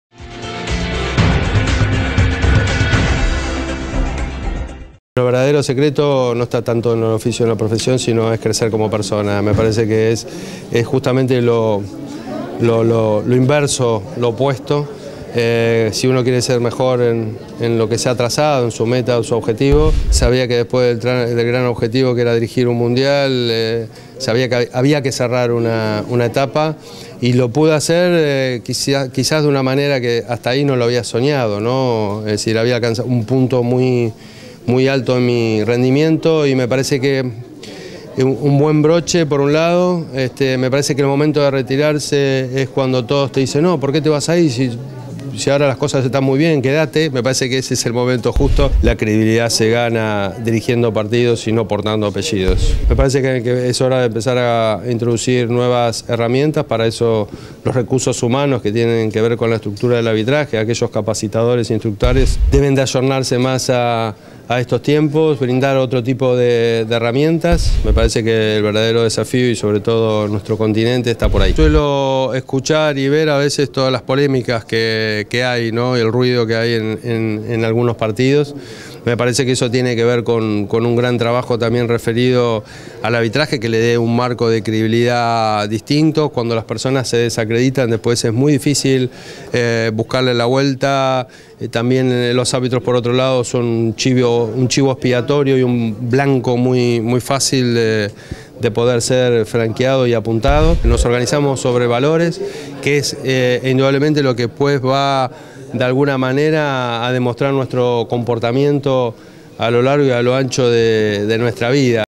Elizondo: "La credibilidad se gana dirigiendo partidos y no portando apellidos"
Horacio Elizondo, ex-árbitro internacional de FIFA, en charla con ABC Tv. Dictará un curso en Paraguay entre el 9 y 10 de junio en el hotel Bourbon sobre "La toma de decisiones" y "El trabajo en equipo".